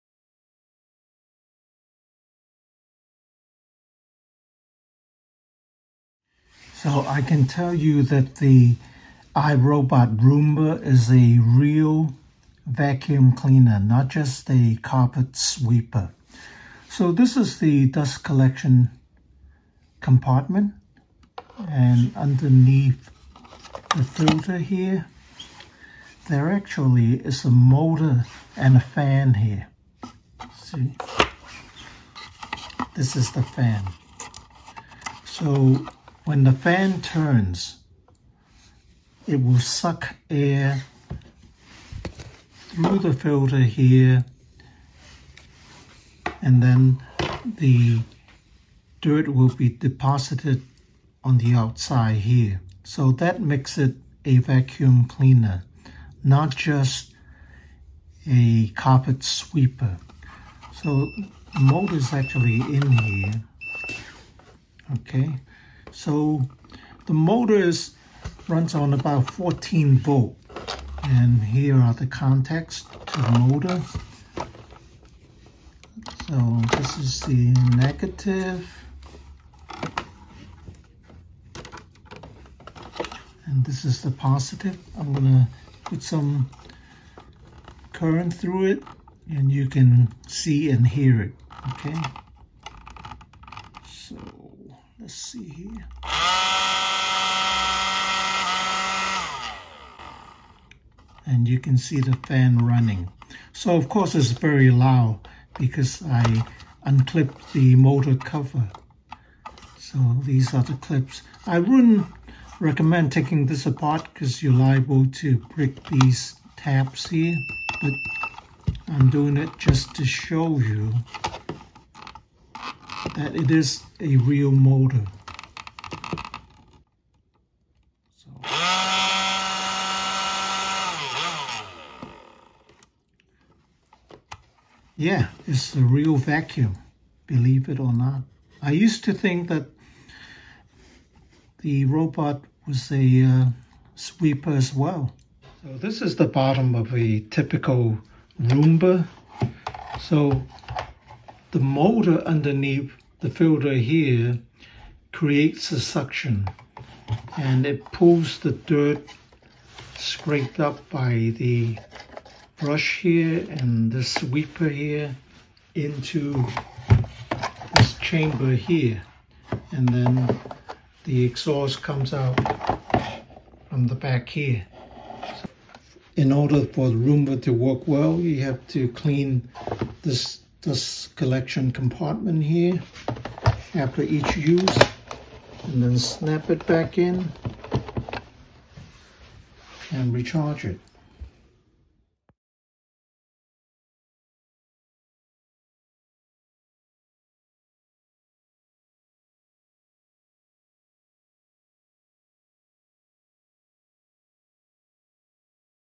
The iRobot Roomba works because it is a true vacuum, not just a carpet sweeper
Some say the Roomba is just a carpet sweeper. That is not true. The iRobot Roomba is an actual vacuum cleaner.
Watch the video to see why the Roomba is a true vacuum.
The 675 model that I owned is superseded by the 694.